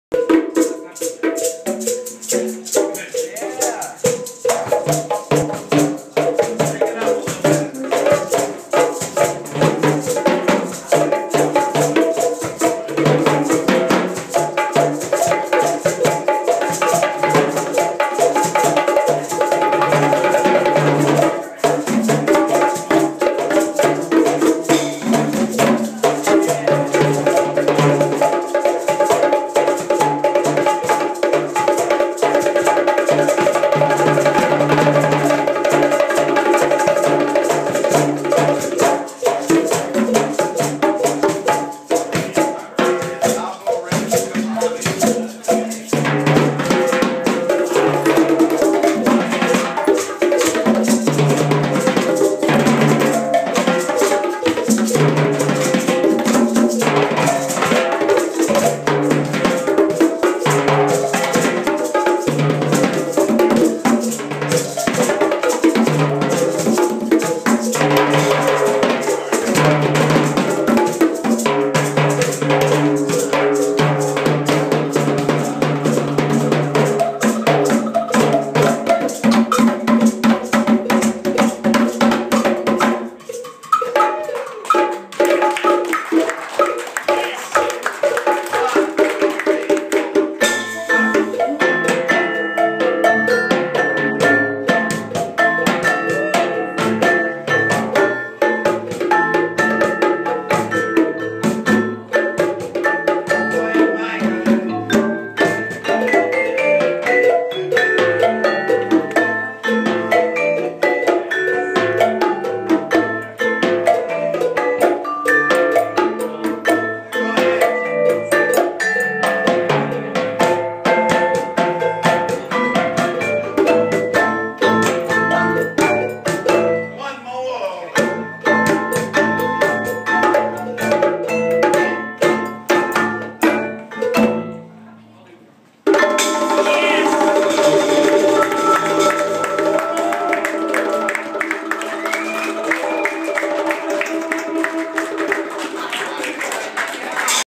Mad bongos at the Pheasant
Some mad drums during a Monday Night Jazz performance by Latin Quarter at the Pheasant in Brookings, South Dakota.